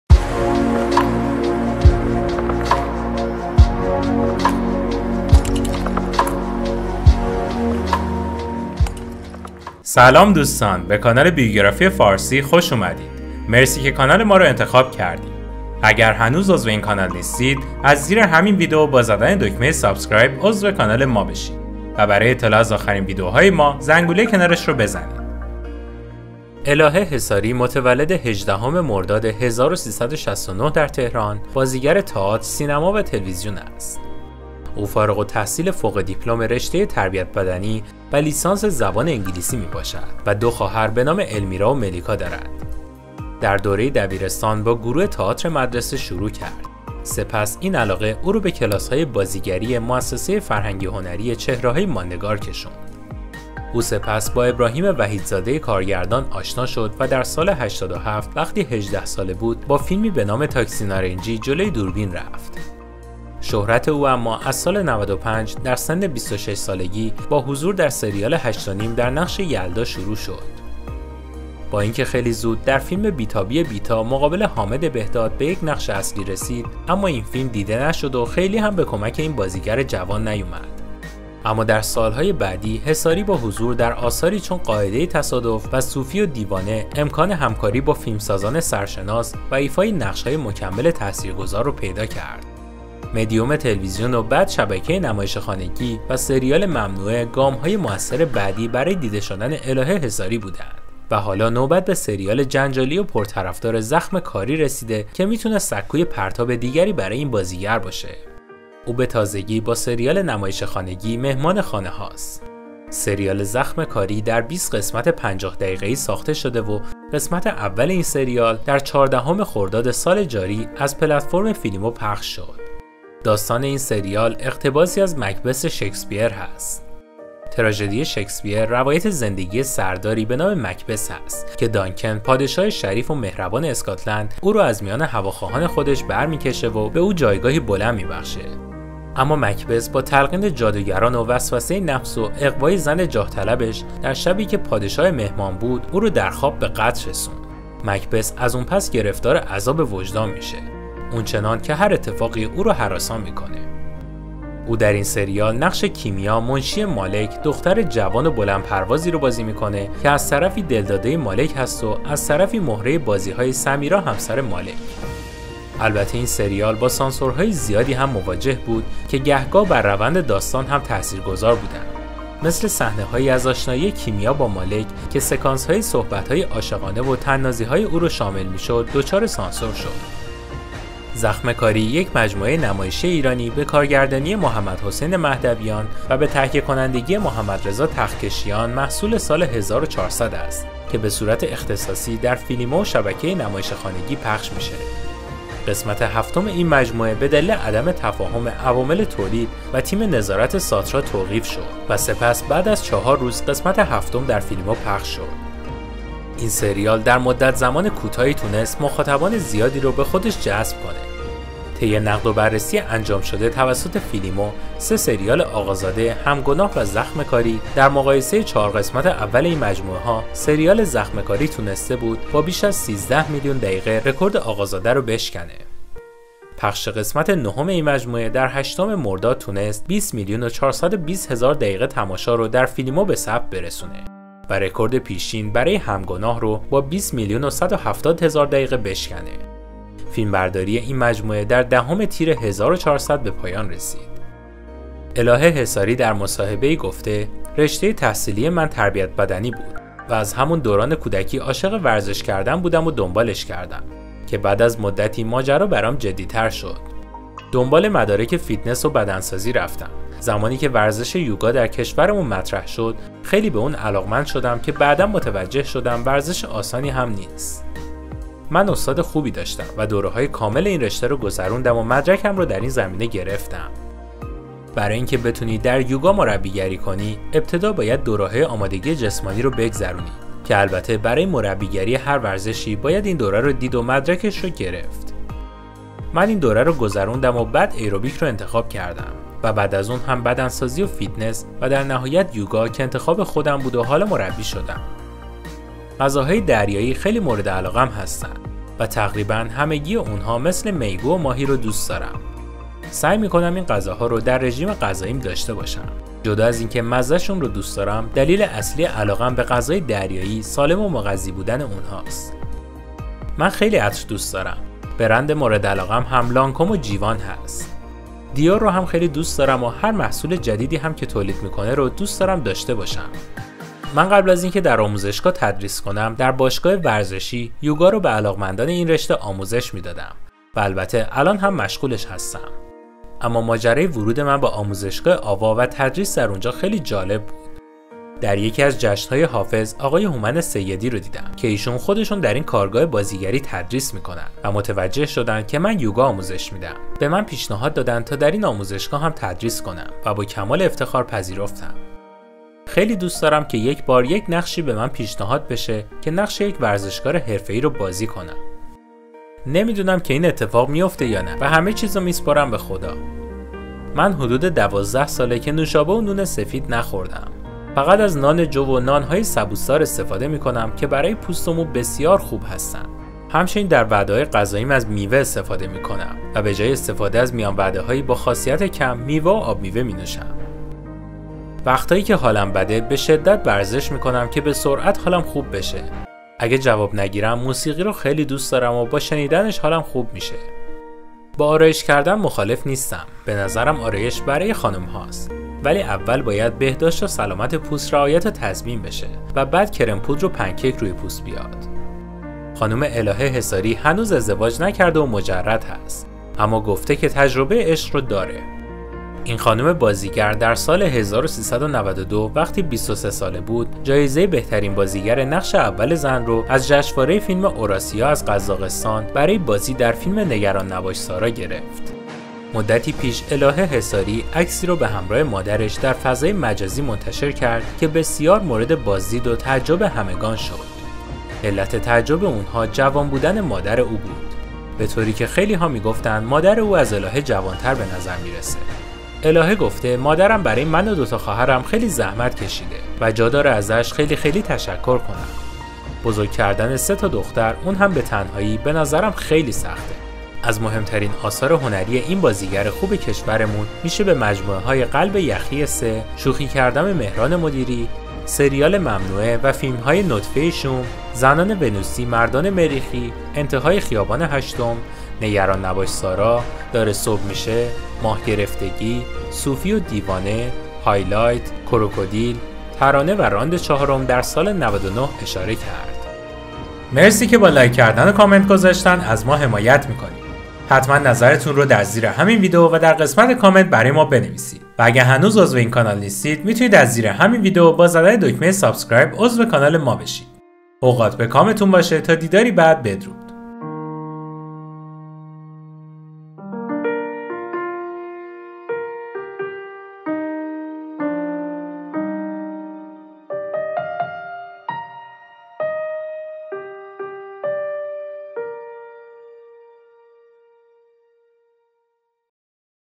بیوگرافی جذاب و شنیدنی الهه حصاری بازیگر سریال زخم کاری
الهه حصاری متولد 18 مرداد 1369 در تهران ، بازیگر سینما و تلویزیون است
فارغ التحصیل فوق دیپلم رشته تربیت بدنی و لیسانس زبان انگلیسی می باشد و دو خواهر بنام المیرا و ملیکا دارد